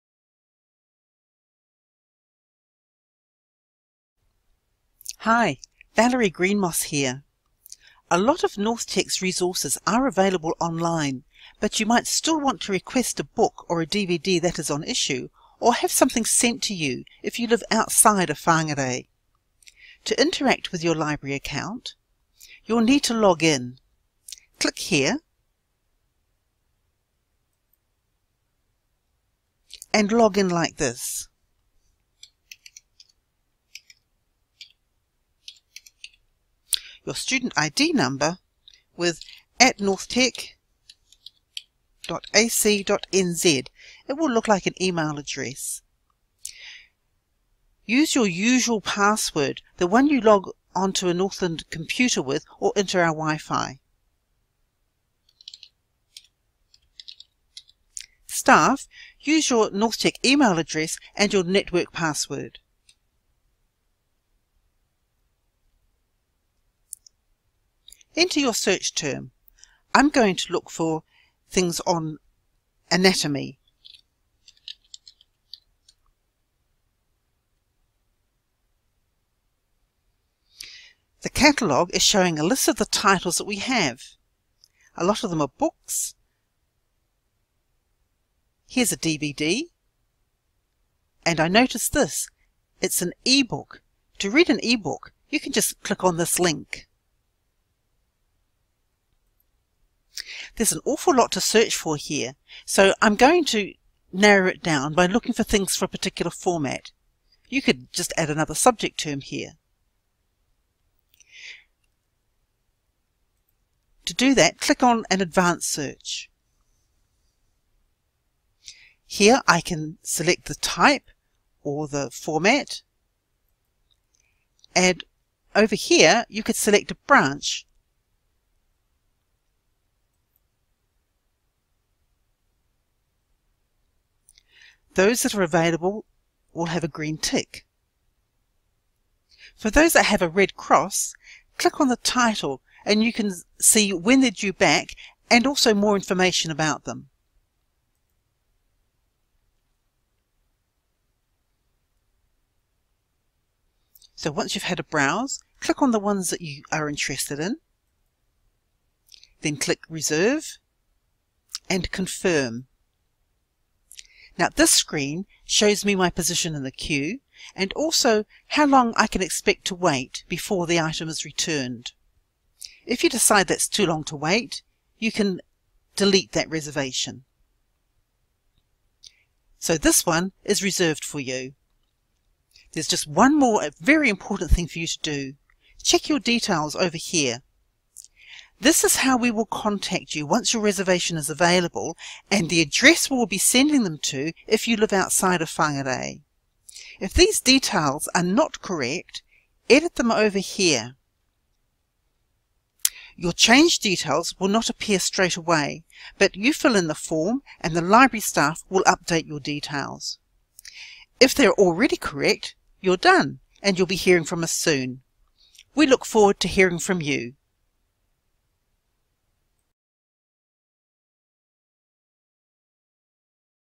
How to reserve and request books and DVD
Valerie shows NorthTec students and staff how to put a hold on a book, and how those based in the regions can have books or DVDs sent to them. For free!